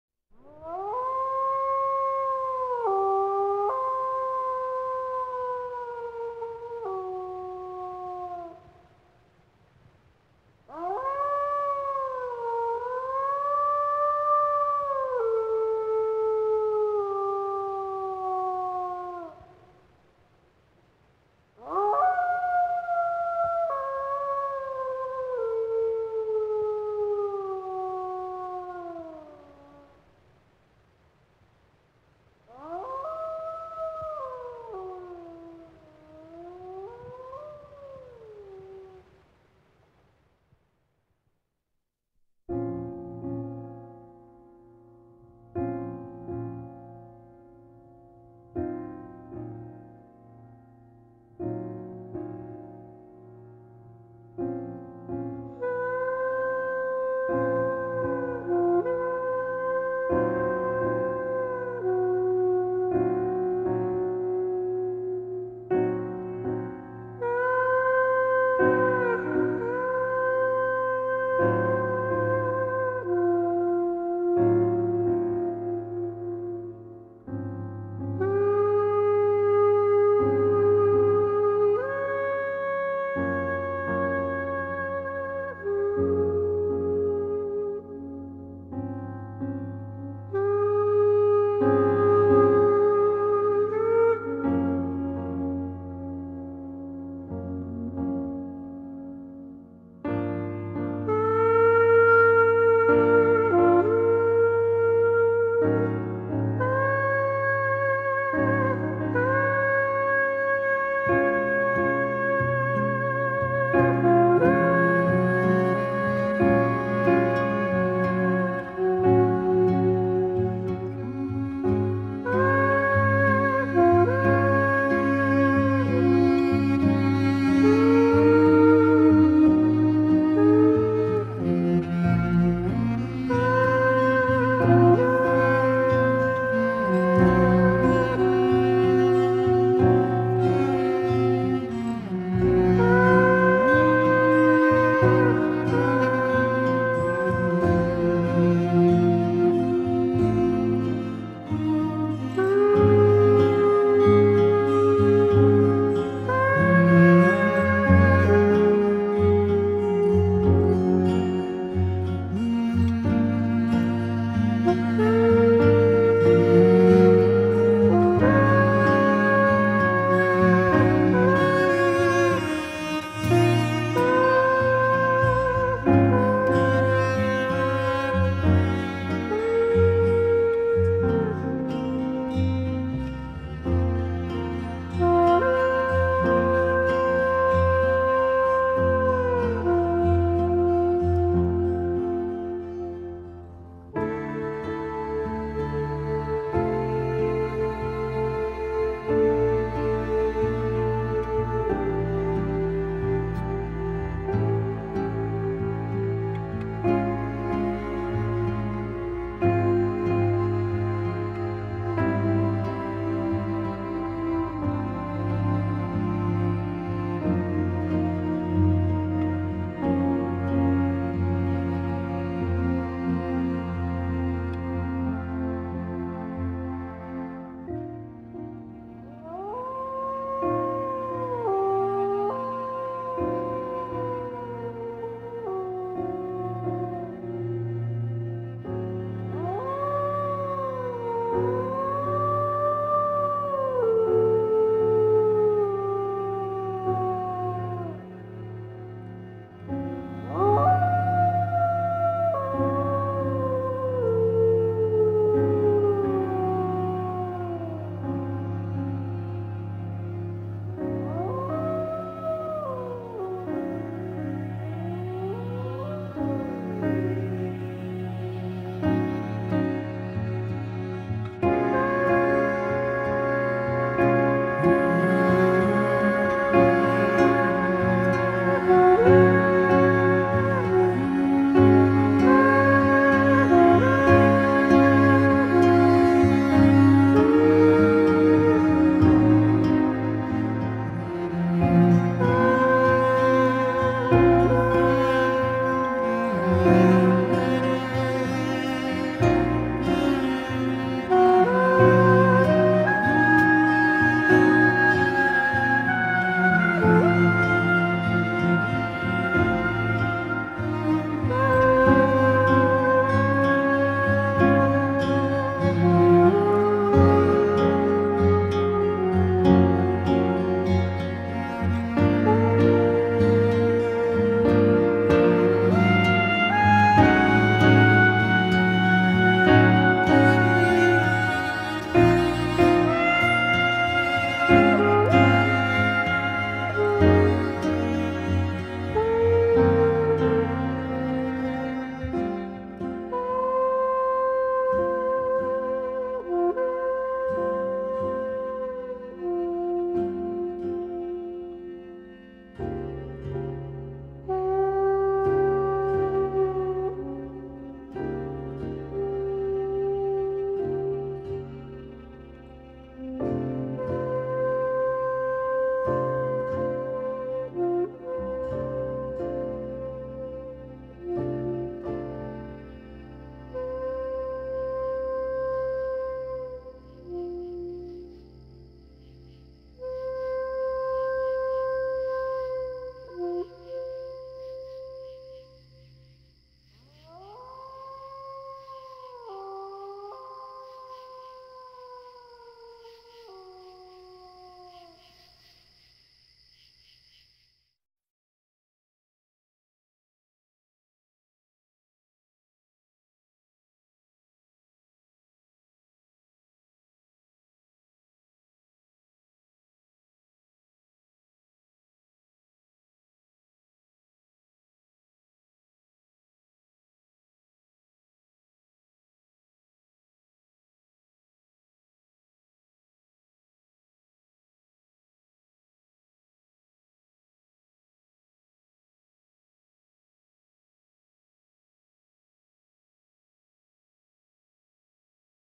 HannahShook1.mpg
Artist Hanna Shook paints to music at the Rocky Mountain Horse Expo, Denver, Colorado, 2011.   Her Wolf Muse joins her as she creates incredible expressionistic paintings with oils and other media.